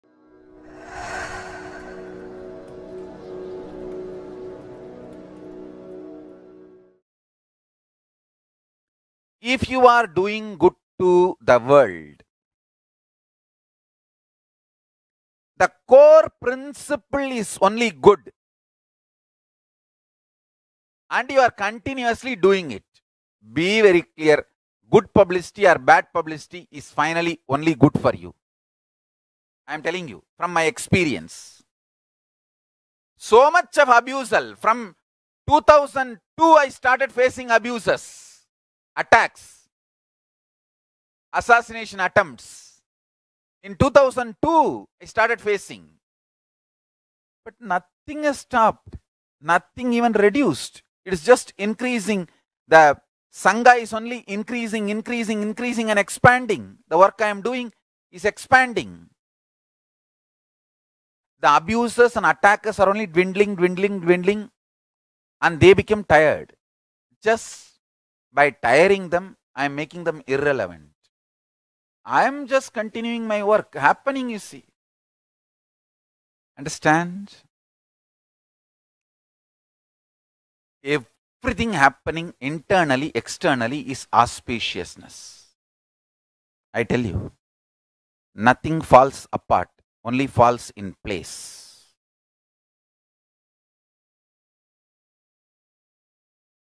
Every Happening is Auspicious
If you are doing good to the world, the core principle is only good and you are continuously doing it, be very clear good publicity or bad publicity is finally only good for you. I am telling you from my experience, so much of abusal, from 2002 I started facing abuses, attacks, assassination attempts, in 2002 I started facing but nothing has stopped, nothing even reduced, it’s just increasing, the Sangha is only increasing, increasing, increasing and expanding, the work I am doing is expanding. The abuses and attacks are only dwindling, dwindling, dwindling, and they became tired. Just by tiring them i am making them irrelevant, I am just continuing my work happening you see. Understand, everything happening internally externally is auspiciousness, I tell you nothing falls apart only falls in place.